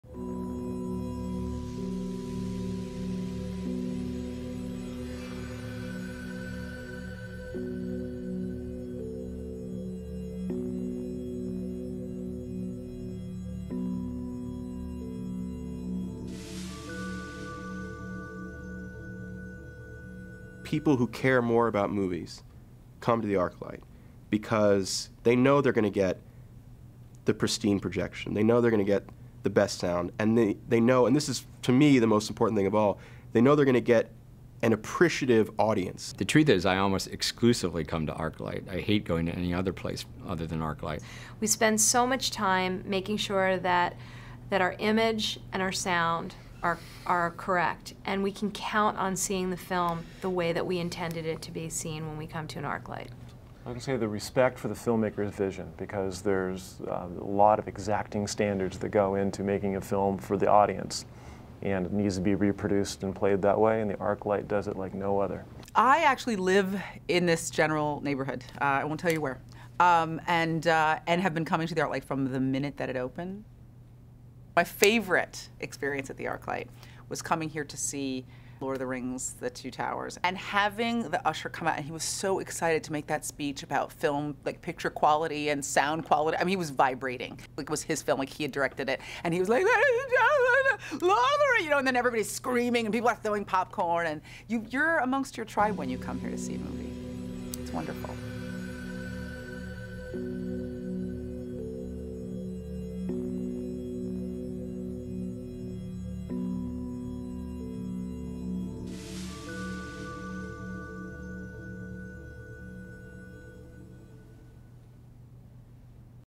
ArcLight Member Video #1
Part of the integrated 10 Year Anniversary campaign for ArcLight Cinemas, ten "ArcLight Member" videos were run prior to movies throughout all of the ArcLight locations.

This video features screenwriter Seth Grahame-Smith (Abraham Lincoln: Vampire Hunter);  screenwriter David Goyer (Batman Begins, Man of Steel): Academy Award nominees, producers Michael Shamberg and Stacey Sher (Erin Brokovich); sound editors Ron Bartlett and Academy Award nominee Mark Stoeckinger (Sound: Star Trek) and Academy Award winner Doug Hemphill (Sound: Last Of The Mohicans) and actress Aisha Tyler (Archer).